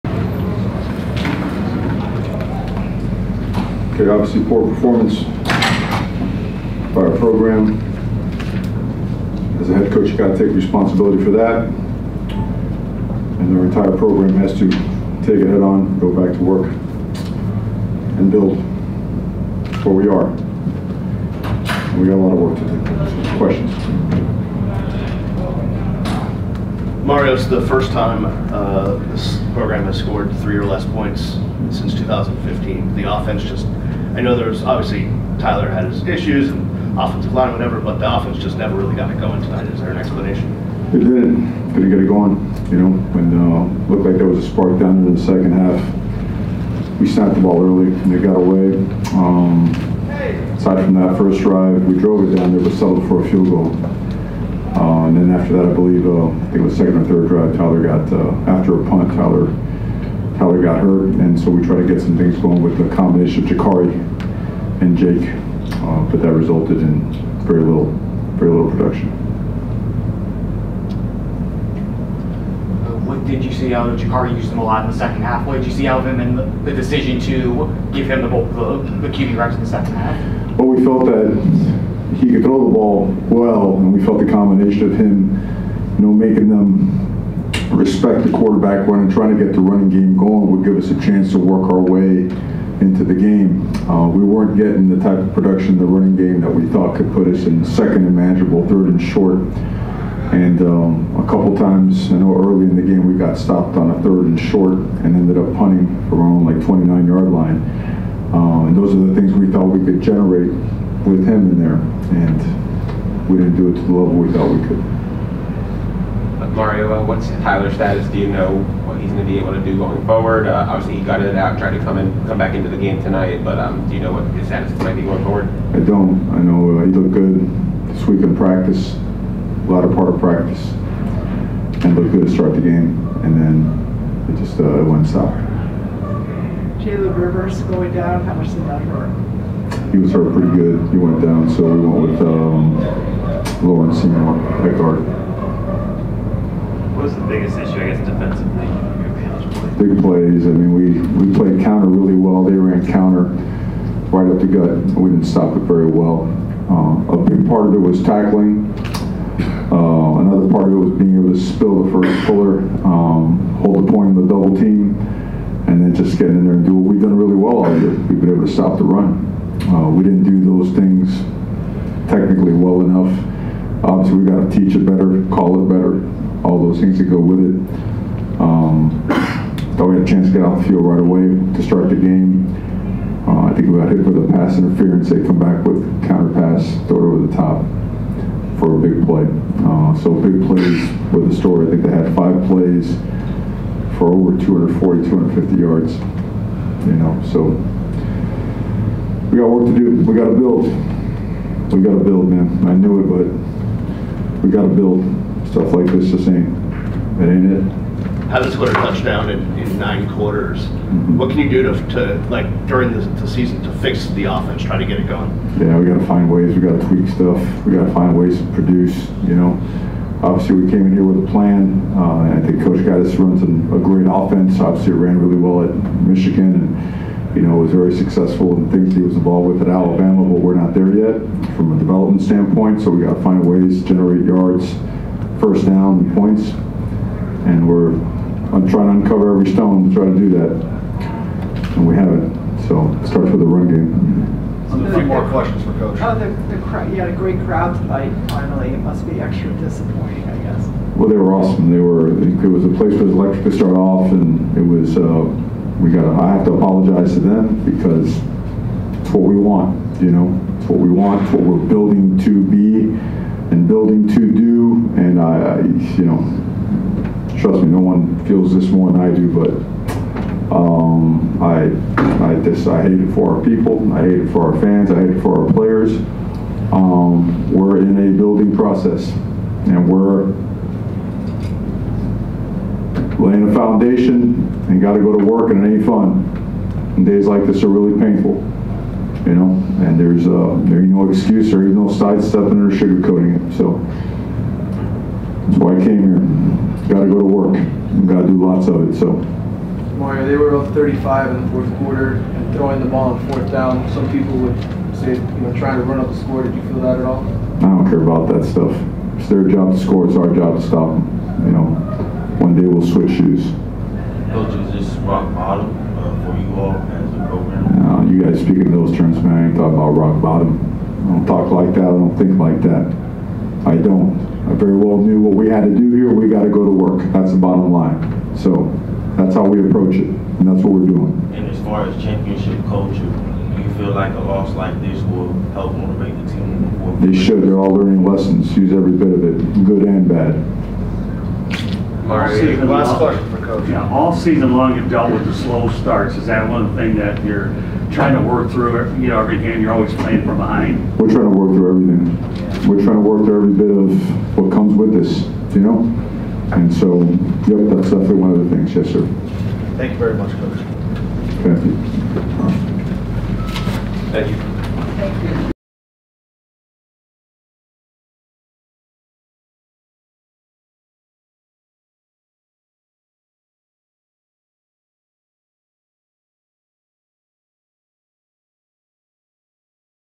Mario Cristobal Postgame Press Conference After 42-Point Loss to FSU "I Hate This For Our People"
Miami Hurricanes head coach Mario Cristobal after a 45-3 loss to Florida State on Nov. 5, 2022.